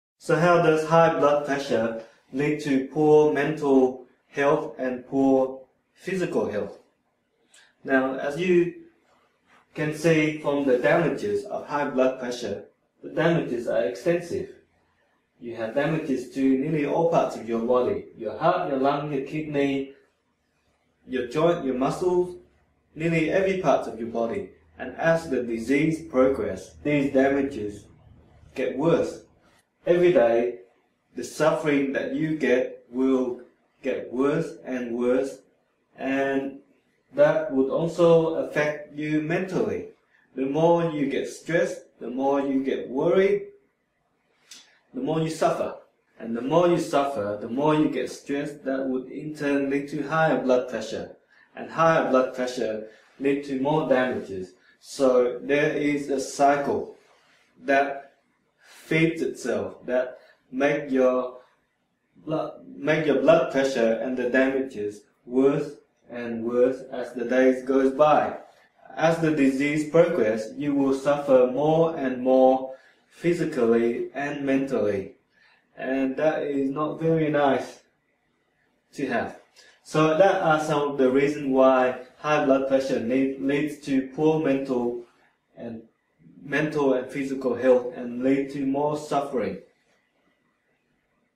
HBP 061- How High Blood Pressure leads to poor mental/physical health
Discover how High Blood Pressure leads to poor mental and physical health.

Please also see our other videos on High Blood Pressure. 67 videos related to this topic has been uploaded on this channel.